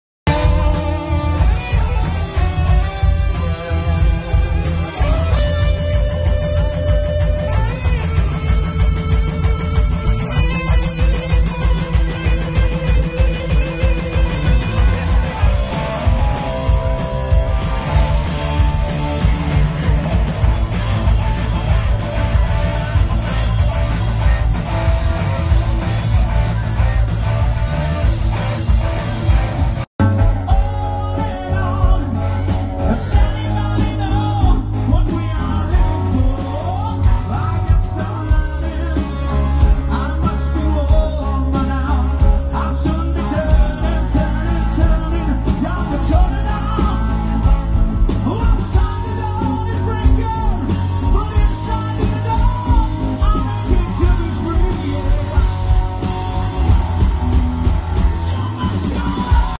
Spider Kartfestivalen 2018
Cover Iron Maiden og Queen medley.
Jan Thore Grefstad
Frode Kolbu
Håkon Kyllingmark
Staale Kaasin (Ståle Kåsin)
Geir Kåre Mykland